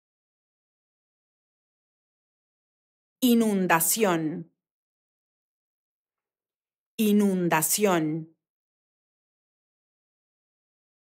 How to pronounce INUNDACIÓN in Latin American Spanish
This video shows you how to pronounce INUNDACIÓN in Latin American Spanish. Speaker has an accent from Lima (Perú). Collins Dictionary, the home of living language and pioneers of dictionary publishing